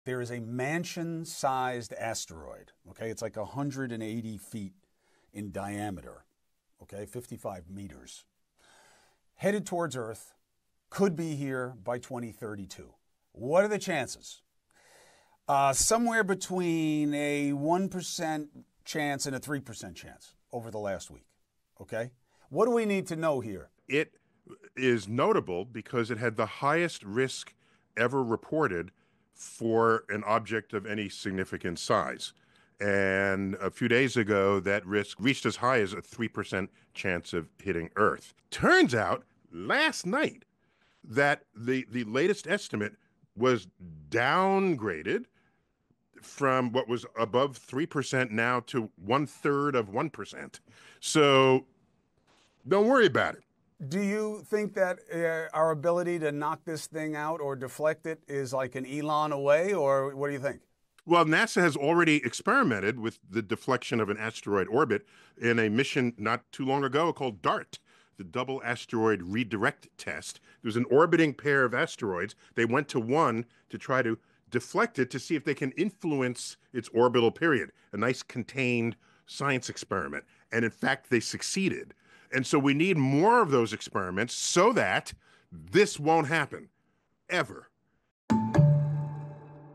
Neil DeGrasse Tyson on how to stop asteroid from hitting earth
An asteroid big enough to wipe out a city could collide with Earth in seven years, according to NASA. Astrophysicist Neil deGrasse Tyson says there are systems in place to deflect asteroids and NASA has run experiments that test its ability to thwart such an event.